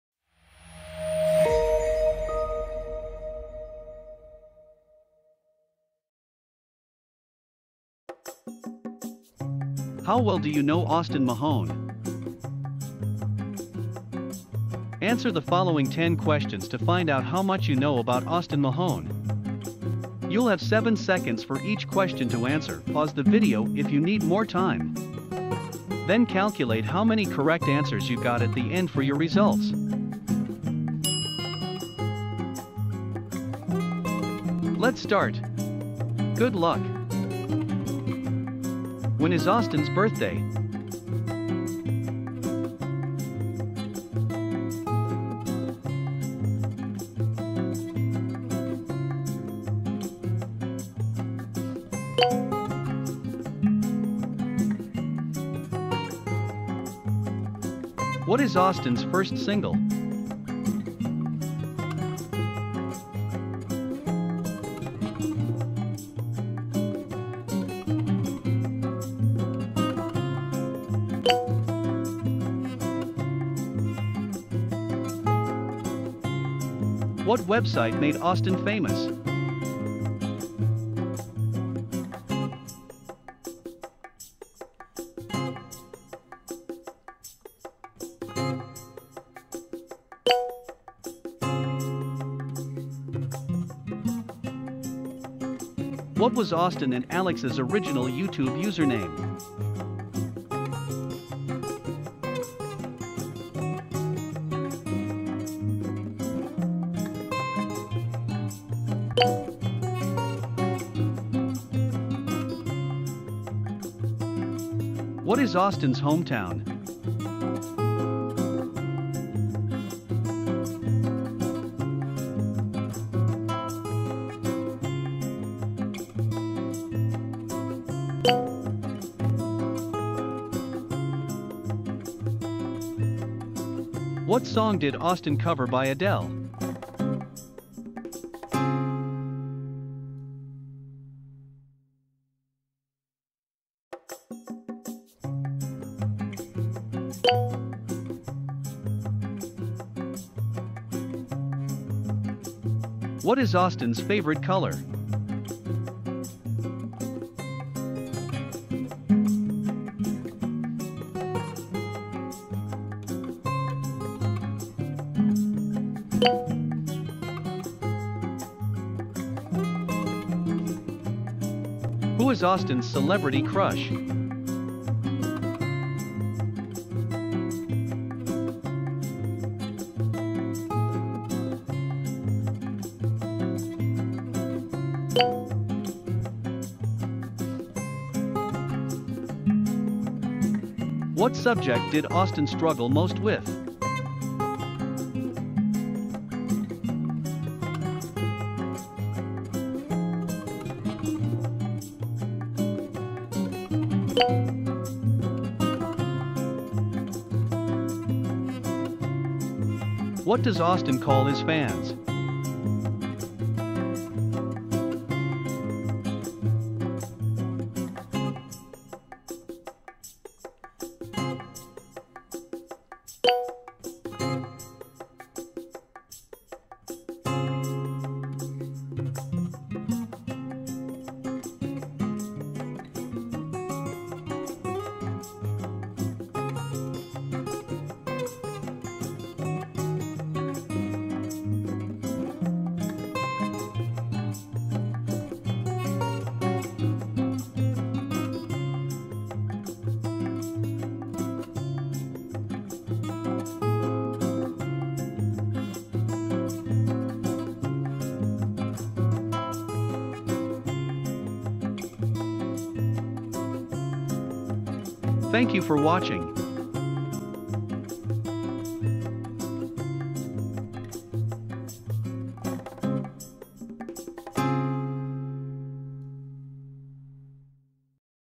How Well Do You Know Austin Mahone Trivia | Singer Quiz #46 | Planet test
Austin Mahone is one of the most famous YouTube sensations and not only is he funny he is also good on the eyes. Austin is a good songwriter and his covers have millions of views. Do you consider yourself one of his fans? If so, then you must know a lot about him and his ventures. Take this quiz and see if you gain the number one spot.